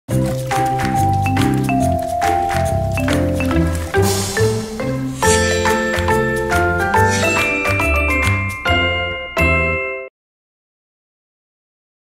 لايفبوي أسرع مما تتخيّل
حماية 99.9% من الجراثيم، مع غسول اليدين سوبر فاست من لايفبوي وفي 10 ثوانٍ فقط.